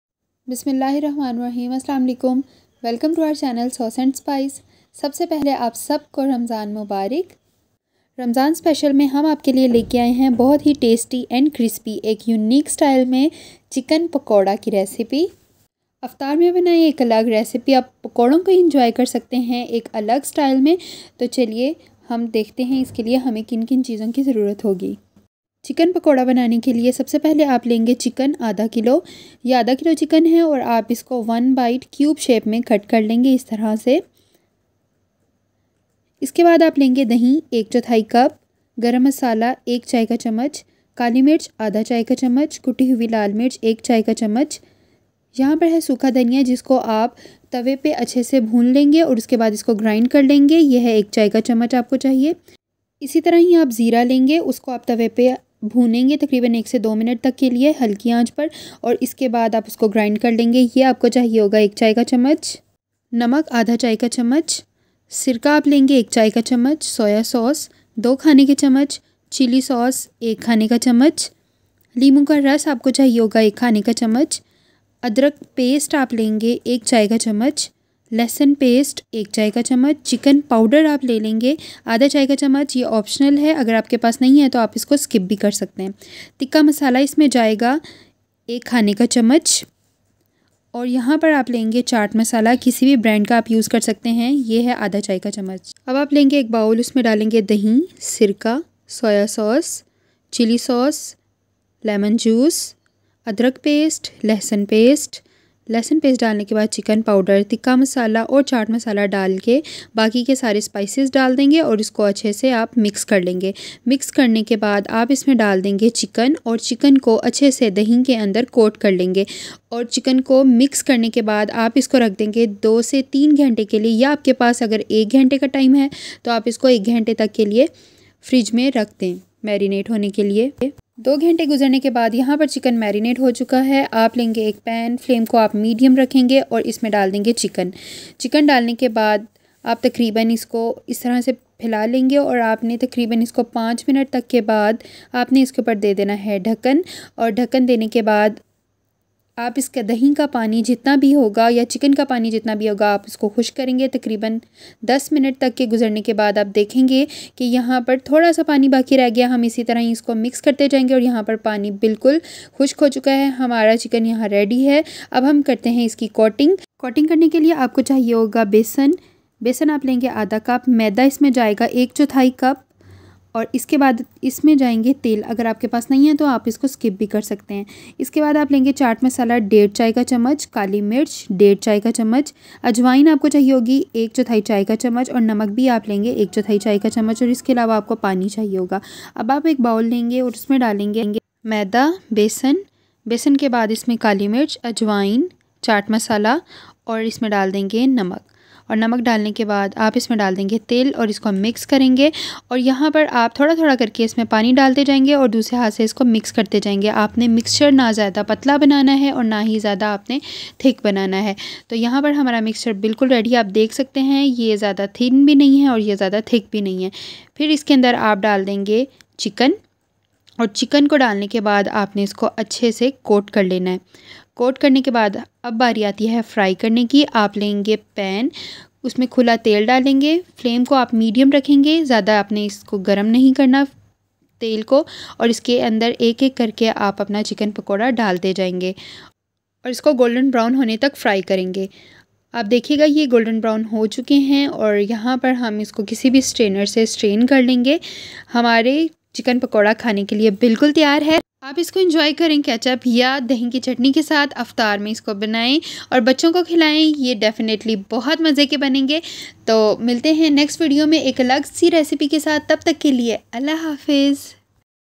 Chicken Tikka Pakora Recipe ( Ramazan Special ) Unique Recipe Easy to make | Sauce and Spice
Hi guys today we are going to share with you a unique and tasty recipe of Chicken Pakora which you can easy make and enjoy in your Iftar time. so don't be late and enjoy it.